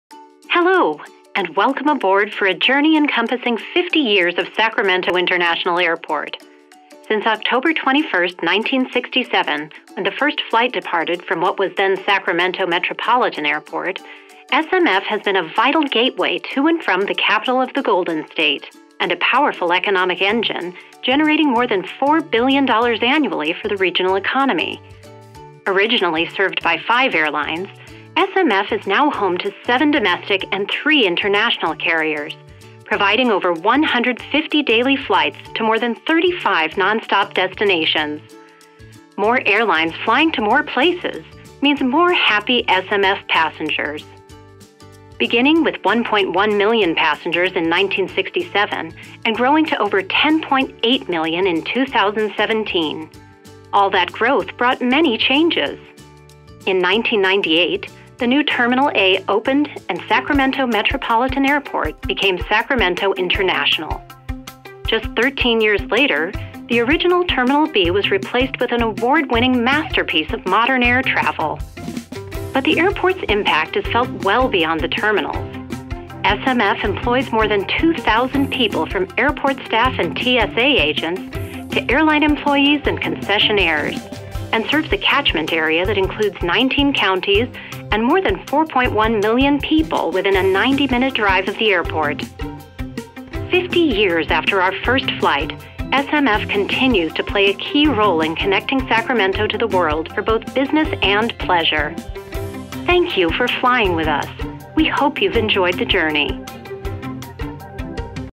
SMF 50 Years!
A fun look at Sacramento International Airport's history over the past 50 years.